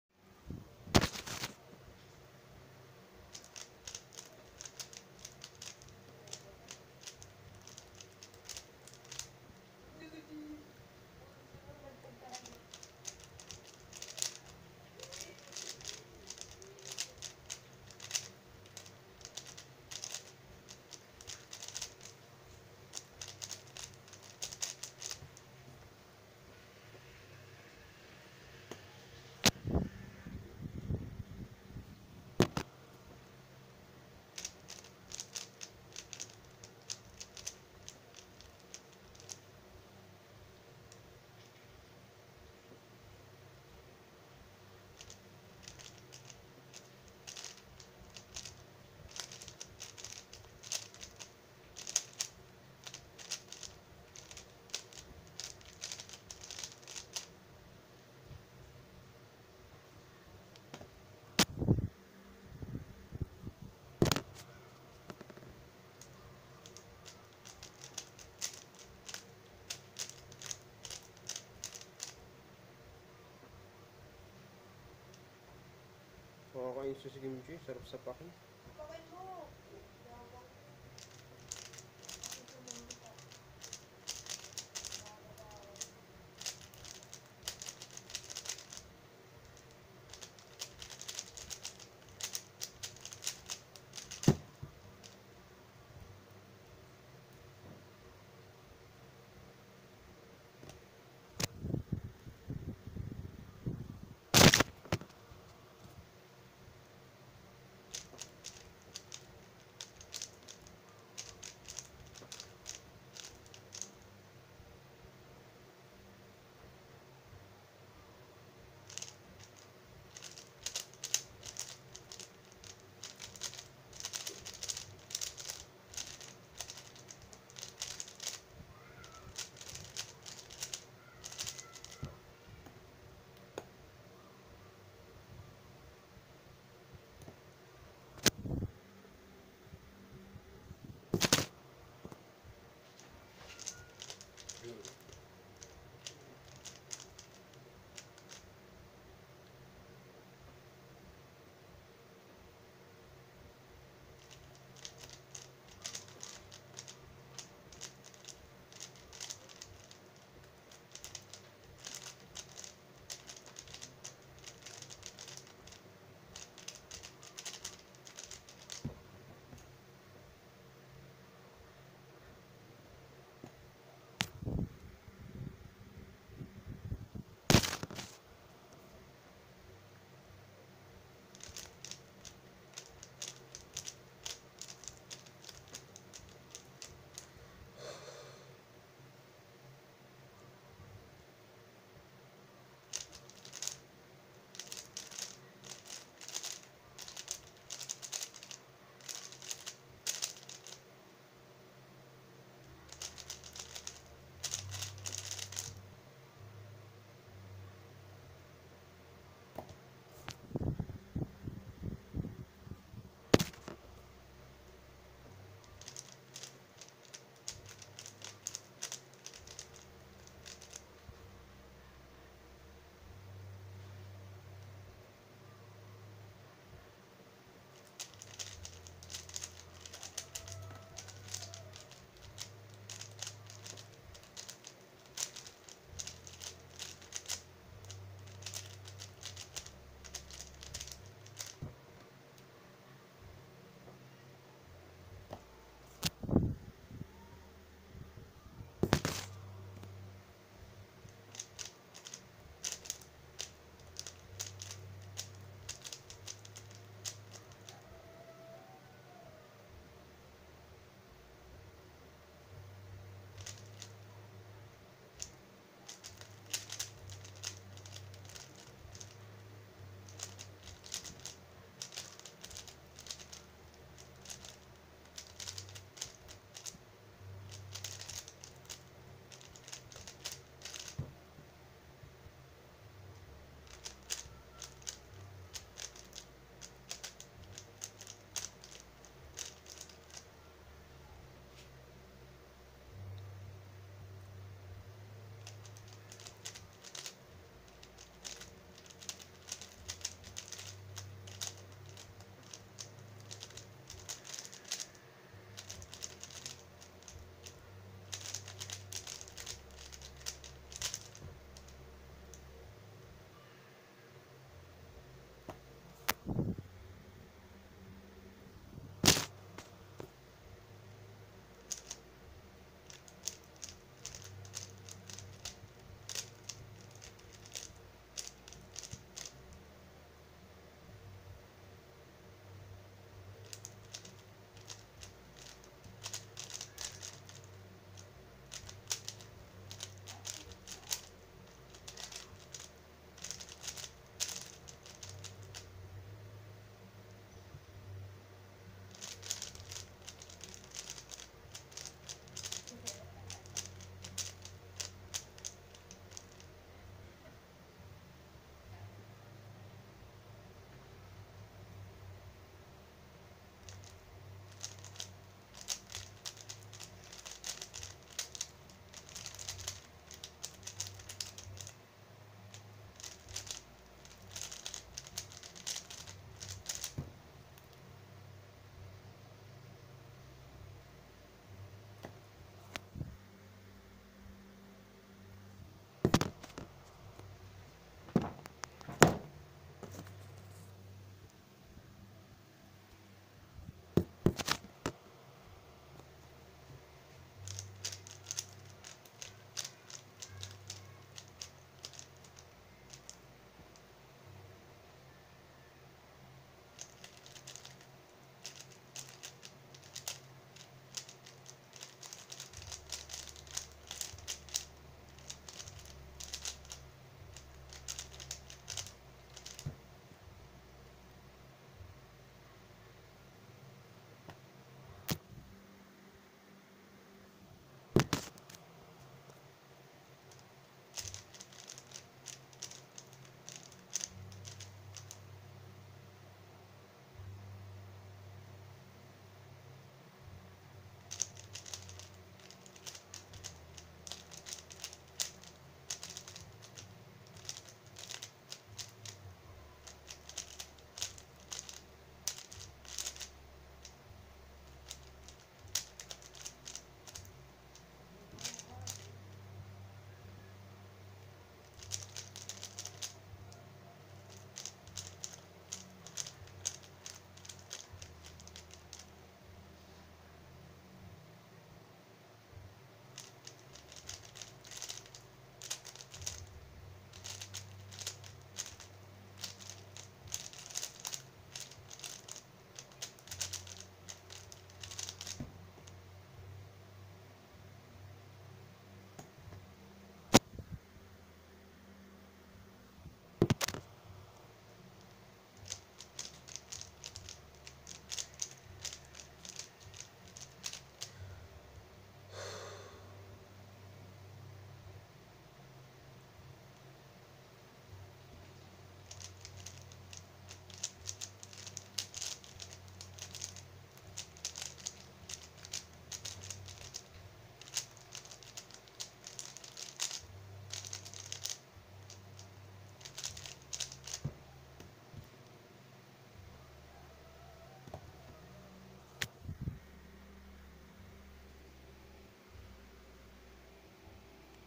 3x3 (avg. Of 12) Part 49 13.69s and PB. 12.39s
1. 13.02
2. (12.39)
3. 13.77
4. 13.36
5. 14.82
6. 12.96
7. 14.03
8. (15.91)
9. 13.54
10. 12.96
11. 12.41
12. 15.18
Final: 13.69s
Cube: Xin Lexin Stickerless Cube
(Blue Chestnuts)